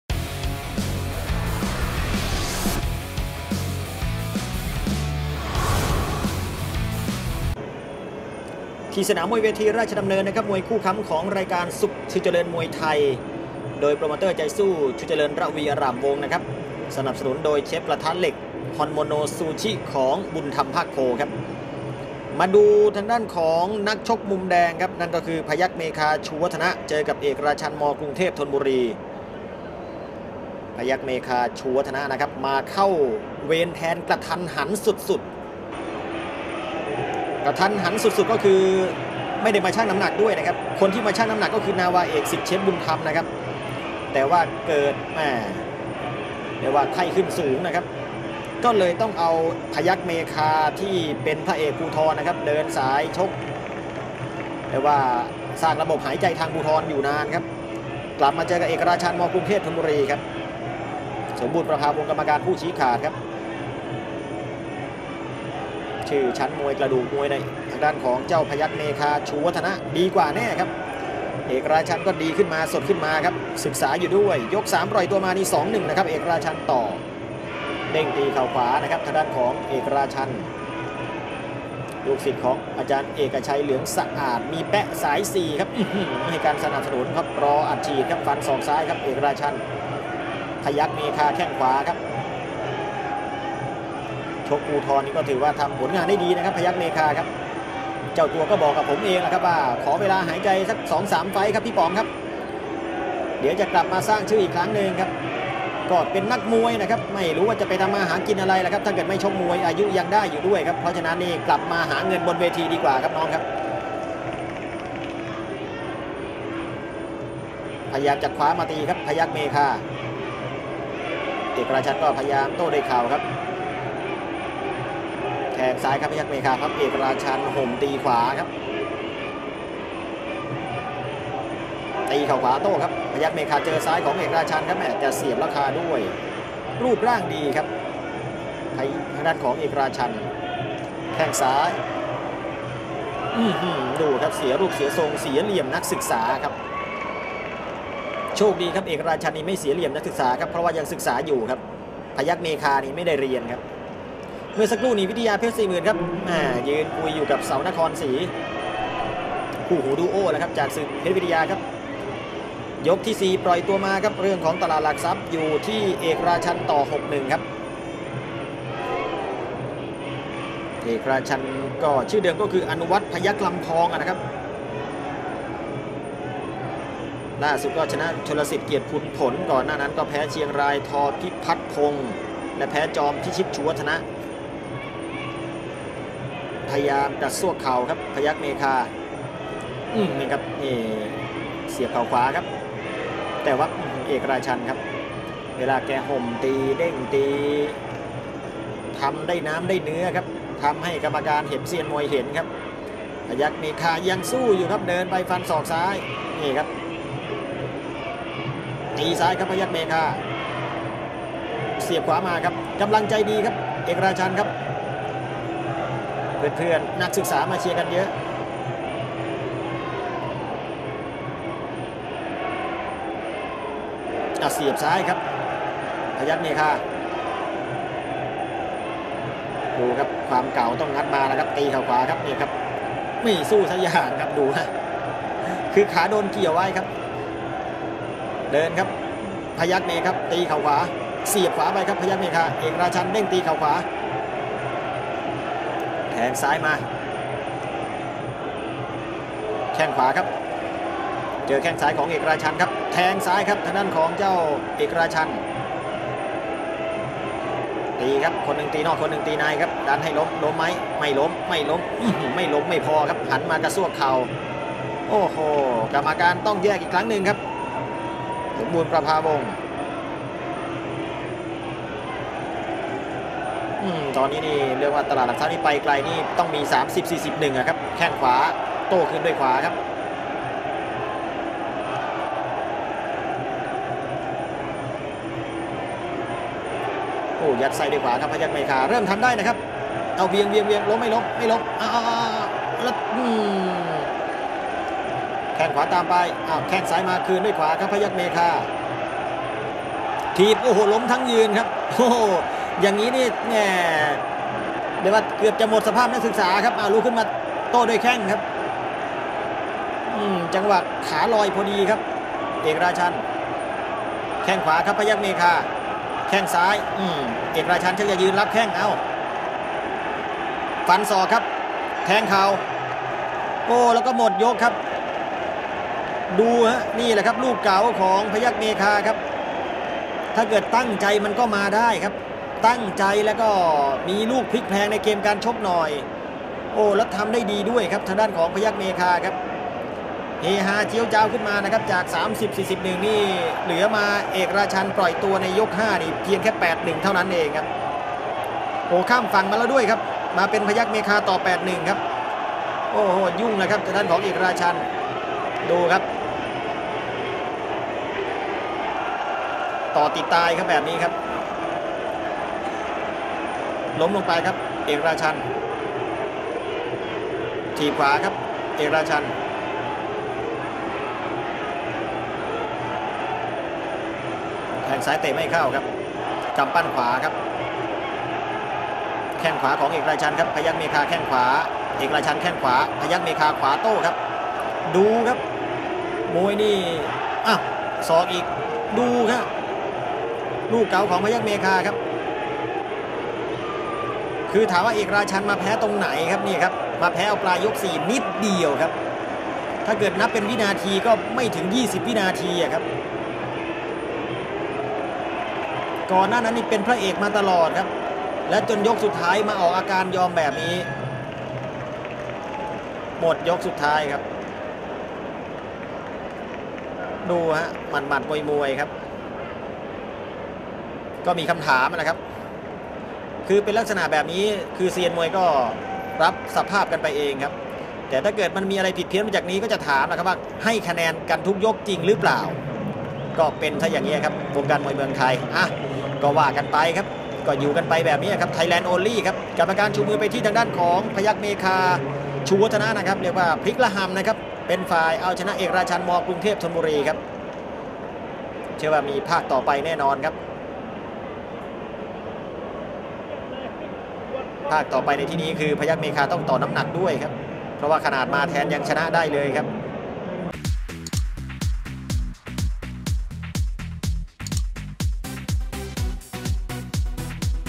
พยัคฆ์เมฆา ชูวัฒนะ VS เอกราชันย์ ม กรุงเทพธนบุรี
ศึกชูเจริญมวยไทย วันที่ 20 เม.ย. 2560 เวทีมวย ราชดำเนิน เวลา 18:30 น.